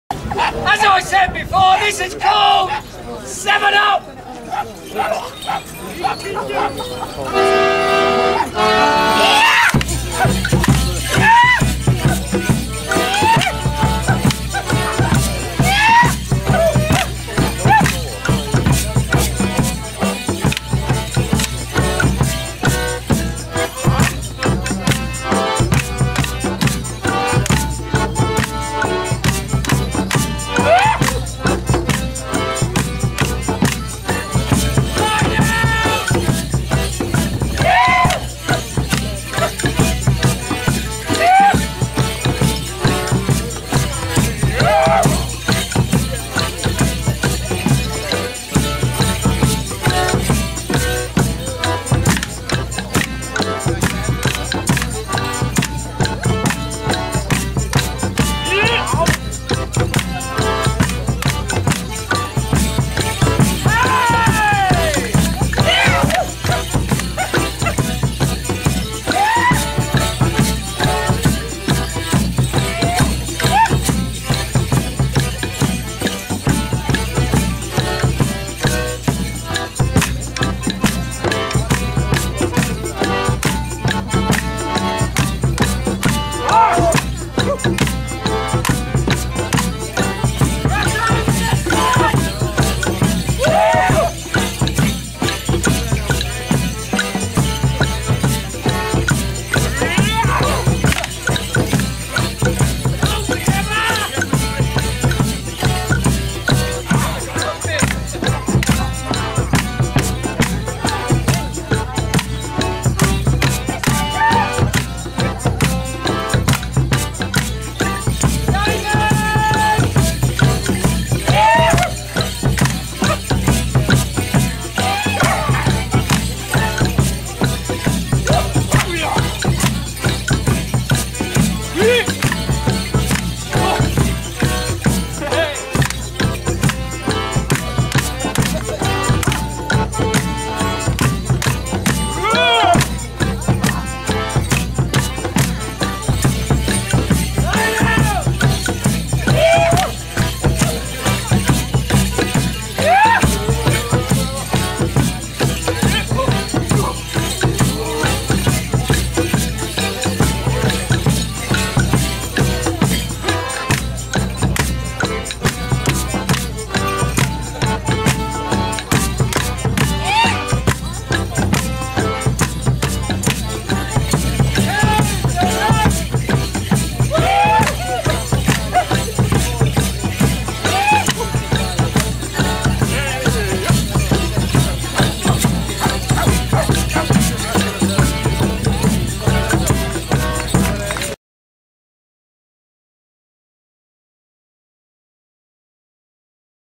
Whitby Folk Week 2011 - Witchmen "Seven Up"
Witchmen dancing at the Cook Monument during Whitby Folk Week 2011. The dance is called Seven Up